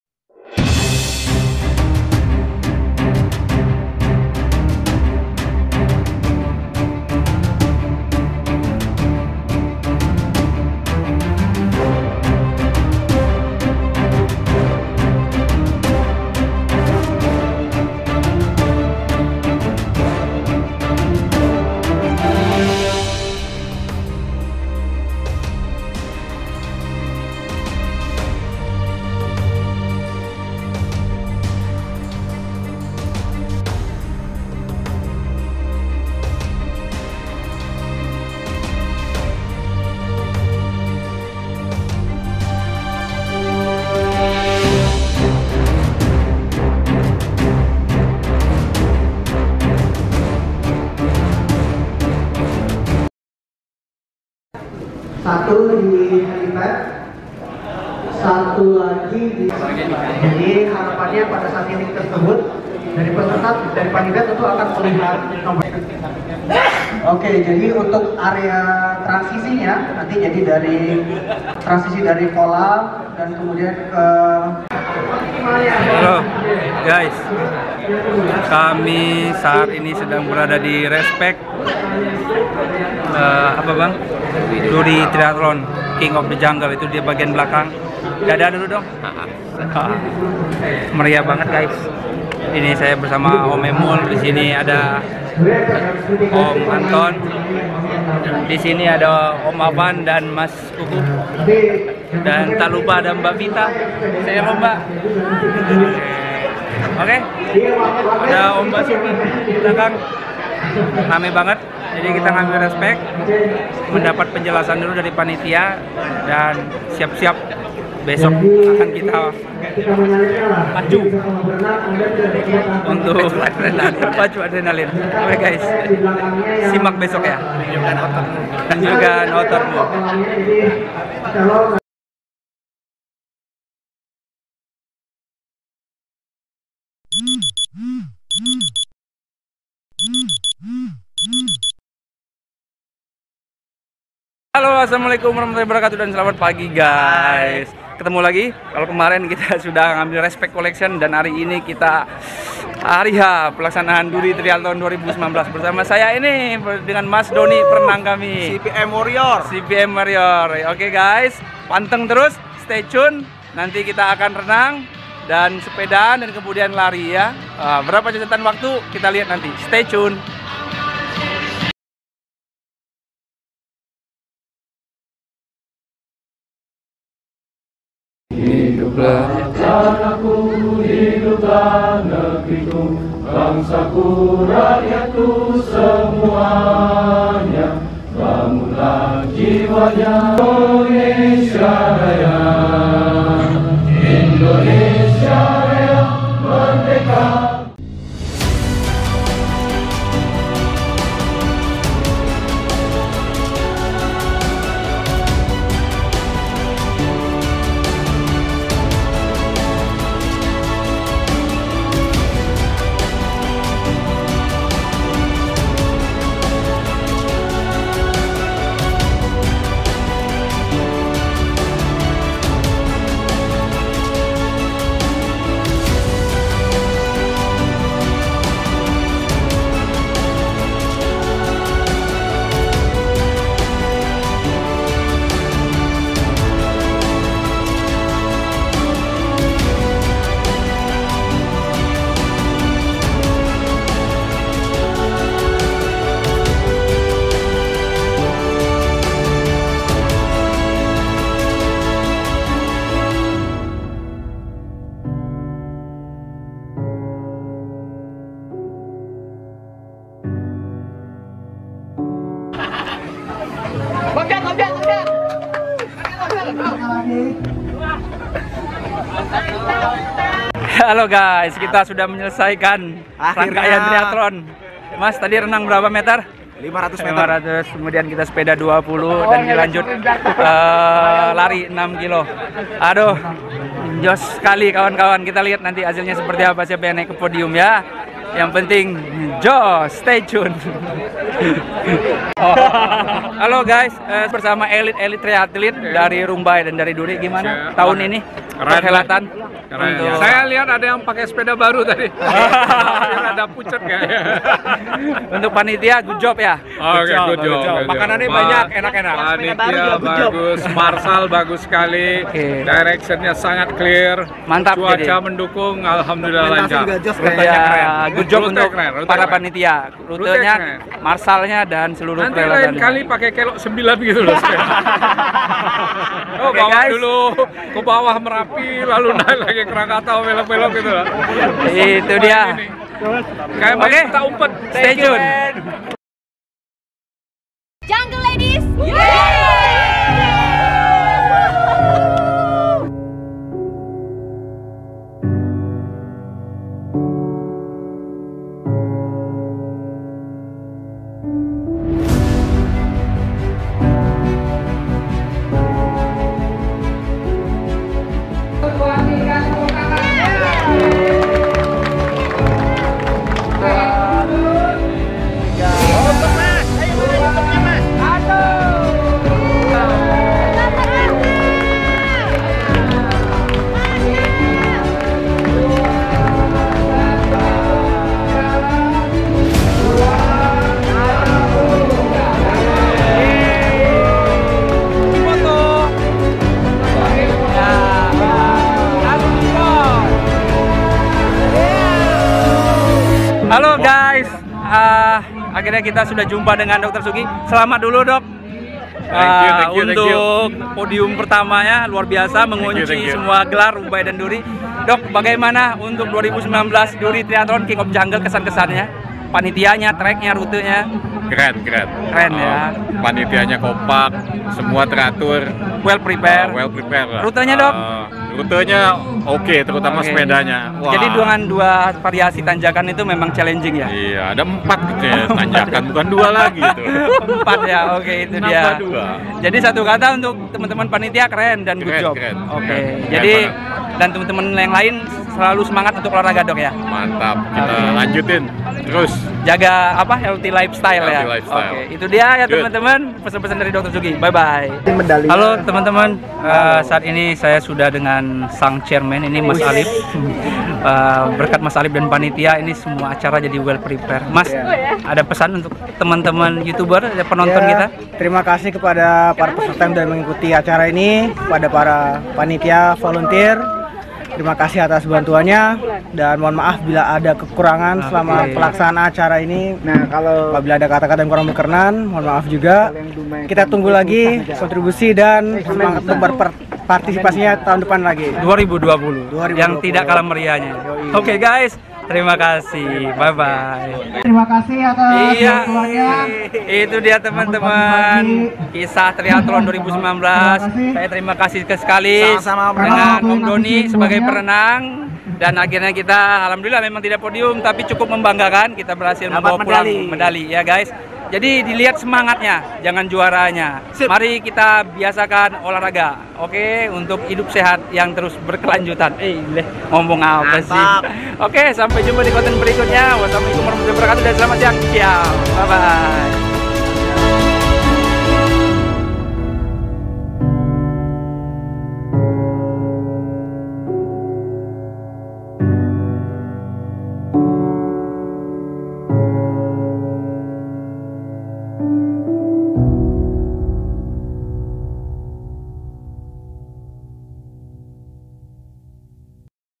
Duri Triathlon 2019 King Of The Jungle | 500M Swim | 20KM Roadbike | 6KM Running
Hi Guys

Please find  Duri Triathlon 2019 documentation.
500  swim combine with 20 KM road bike also 6 KM running.
Hope you like guys and inspiring you to keep exercise out there.
Many thanks to all committee for your good job and well prepared event.

Dip Dip Band Project #136